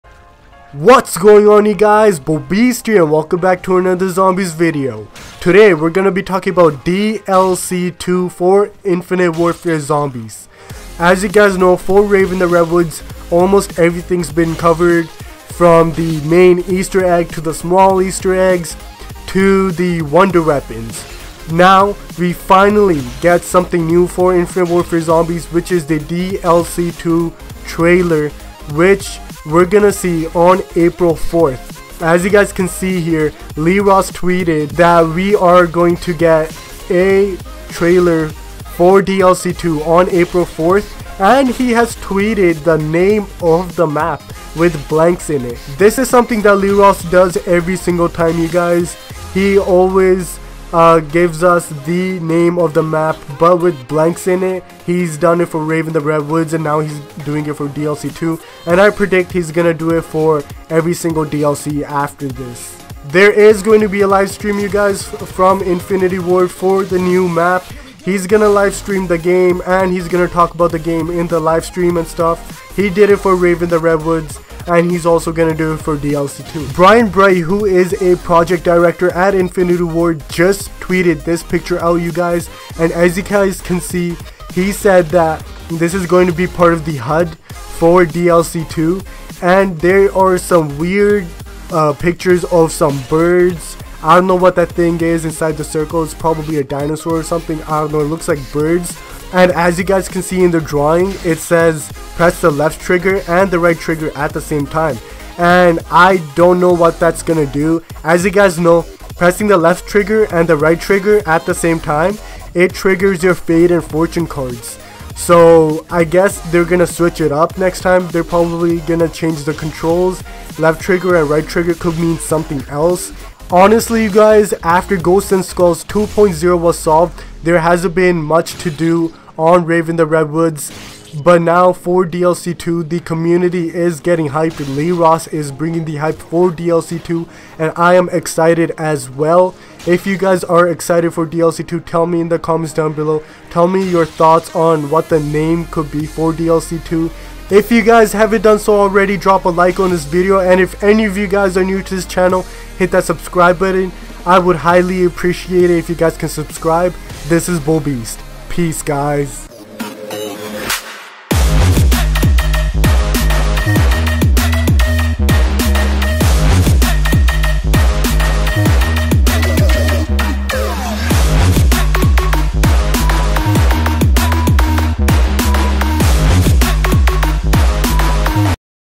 HUGE LEE ROSS DLC 2 HINTS AND TRAILER REVEAL DATE! (INFINITE WARFARE)
HUGE LEE ROSS DLC 2 HINTS AND TRAILER REVEAL DATE! (INFINITE WARFARE). Were gonna be looking at the hints that Lee Ross is giving us about DLC 2 long with some other things.

ÉWN